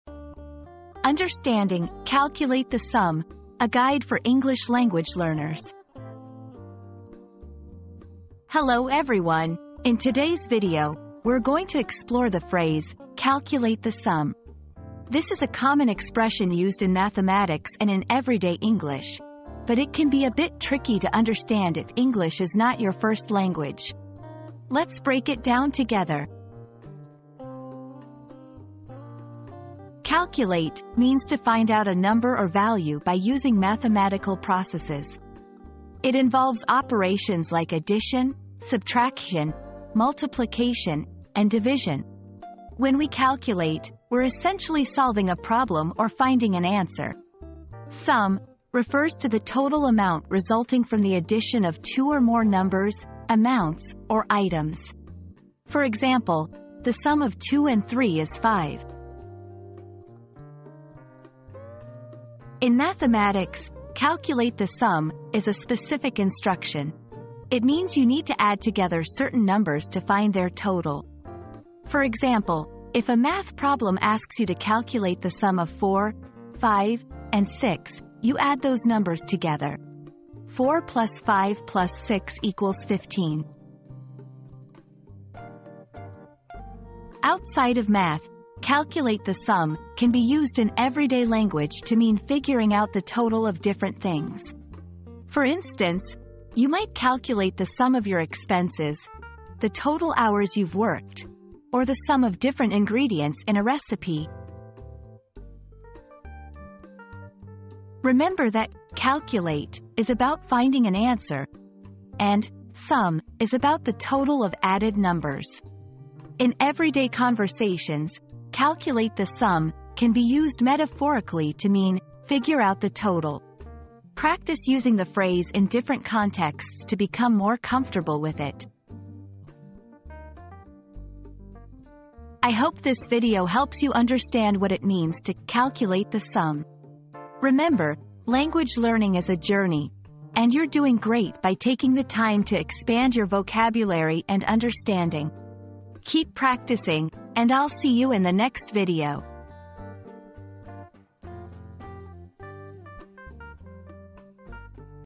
Understanding "Calculate the Sum": A Guide for English Language Learners
00:00 • Introduction - Understanding "Calculate the Sum": A Guide for English Language Learners
00:29 • Understanding the Terms
01:02 • Using "Calculate the Sum" in Mathematics
01:26 • "Calculate the Sum" in Everyday Language
01:47 • Tips for Remembering and Using the Phrase